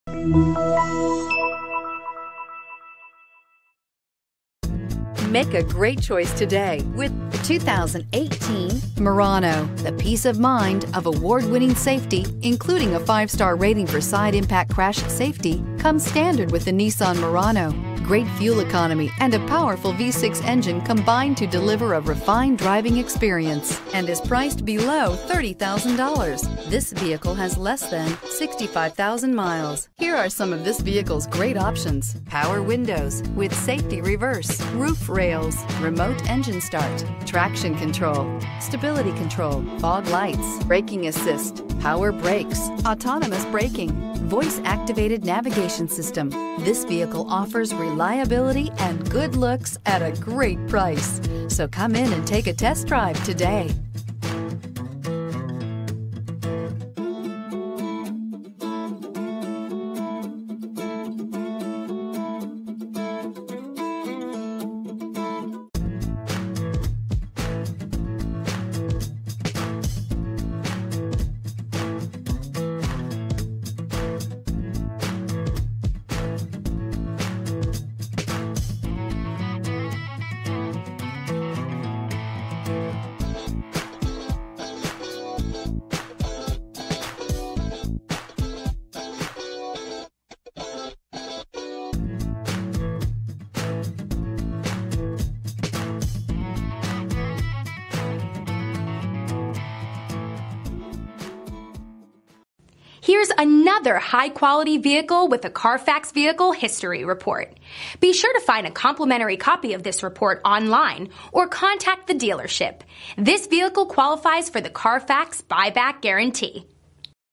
2018 Nissan Murano P10952B
2018 Nissan Murano SL

When you're ready to upgrade your ride, opt for this 2018 Nissan Murano SL, which includes features such as push button start, remote starter, blind spot sensors, a navigation system, braking assist, dual climate control, a power outlet, hill start assist, stability control, and traction control.  It comes with a 6 Cylinder engine.  We've got it for $25,758.  This one's a keeper. It has a crash test safety rating of 4 out of 5 stars.  For a good-looking vehicle from the inside out, this SUV features a classic pearl white exterior along with a graphite interior.  This is the vehicle you have been waiting for!